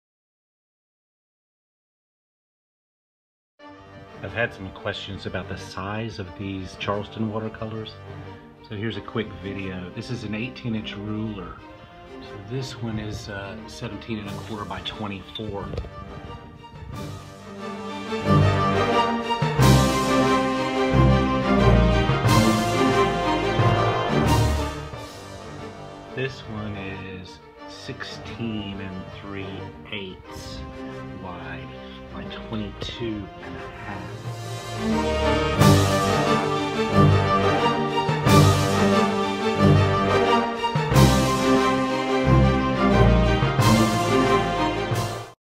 Charleston Watercolors
Sizes of the recent published Charleston watercolors, for Dianne and a few others who have asked about sizes.